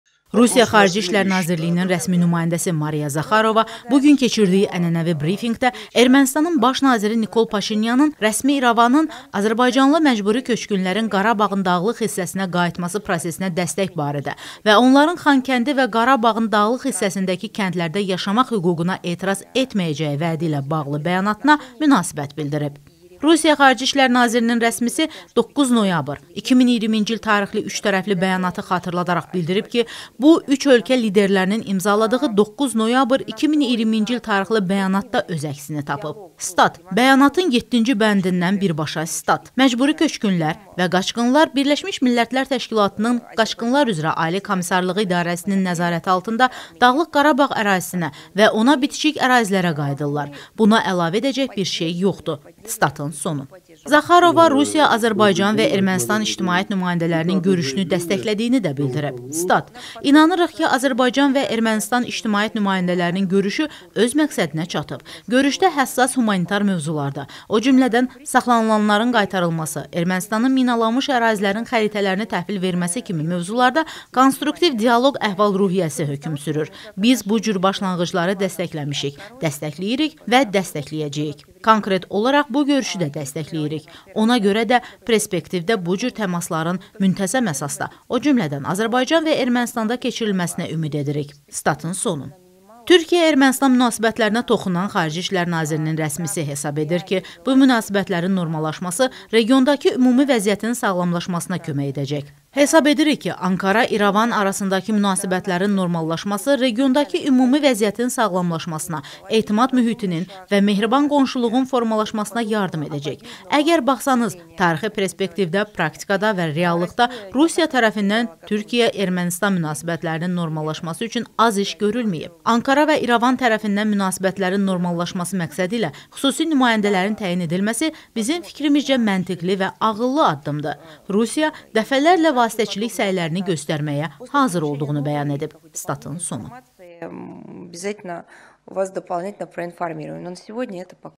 Mariya Zaxarova keçmiş azərbaycanlı məcburi köçkünlərin qayıdışı prosesindən danışıb
Rusiya Xarici İşlər Nazirliyinin (XİN) rəsmi nümayəndəsi Mariya Zaxarova bu gün keçirdiyi ənənəvi brifinqdə Ermənistanın Baş naziri Nikol Paşinyanın rəsmi İrəvanın azərbaycanlı məcburi köçkünlərin Qarabağın dağlıq hissəsinə qayıtması prosesinə dəstək barədə və onların Xankəndi və Qarabağın dağlıq hissəsindəki kəndlərdə yaşamaq hüququna etiraz etməyəcəyi vədi ilə bağlı bəyanatına münasibət bildirib.
Rusiya XİN rəsmisi 9 noyabr 2020-ci il tarixli üçtərəfli bəyanatı xatırladaraq bildirib ki, bu, üç ölkə liderinin imzaladığı 9 noyabr 2020-ci il tarixli bəyanatda öz əksini tapıb.
“Bəyanatın 7-ci bəndindən birbaşa sitat: “Məcburi köçkünlər və qaçqınlar Birləşmiş Millətlər Təşkilatının (BMT) Qaçqınlar üzrə Ali Komissarlığı İdarəsinin nəzarəti altında Dağlıq Qarabağ ərazisinə və ona bitişik ərazilərə qayıdırlar”. Buna əlavə edəcək bir şey yoxdur”, - deyə XİN rəsmisi vurğulayıb.
Zaxarova Rusiya Azərbaycan və Ermənistan ictimaiyyət nümayəndələrinin görüşünü dəstəklədiyini də bildirib.
“İnanırıq ki, Azərbaycan və Ermənistan ictimaiyyət nümayəndələrinin görüşü öz məqsədinə çatıb. Görüşdə həssas humanitar mövzularda, o cümlədən saxlanılanların qaytarılması, Ermənistanın minalanmış ərazilərin xəritələrini təhvil verməsi kimi mövzularda konstruktiv dialoq əhval-ruhiyyəsi hökm sürüb. Biz bu cür başlanğıcları dəstəkləmişik, dəstəkləyirik və dəstəkləyəcəyik. Konkret olaraq bu görüşü də dəstəkləyirik. Ona görə də perspektivdə bu cür təmasların müntəzəm əsasda, o cümlədən Azərbaycan və Ermənistanda keçirilməsinə ümid edirik”.
Türkiyə-Ermənistan münasibətlərinə toxunan XİN rəsmisi hesab edir ki , bu münasibətlərin normallaşması regiondakı ümumi vəziyyətin sağlamlaşmasına kömək edəcək.

 “Hesab edirik ki, Ankara-İrəvan arasındakı münasibətlərinin normallaşması regiondakı ümumi vəziyyətin sağlamlaşmasına, etimad mühitinin və mehriban qonşuluğun formalaşmasına yardım edərdi. Əgər baxsanız, tarixi perspektivdə, praktikada və reallıqda Rusiya tərəfindən Türkiyə-Ermənistan münasibətlərinin normallaşması üçün az iş görülməyib. Ankara və İrəvan tərəfindən münasibətlərin normallaşması məqsədi ilə xüsusi nümayəndələrin təyin edilməsi bizim fikrimizcə, məntiqli və ağıllı addımdır. Rusiya dəfələrlə vasitəçilik səylərini göstərməyə hazır olduğunu bəyan edib”.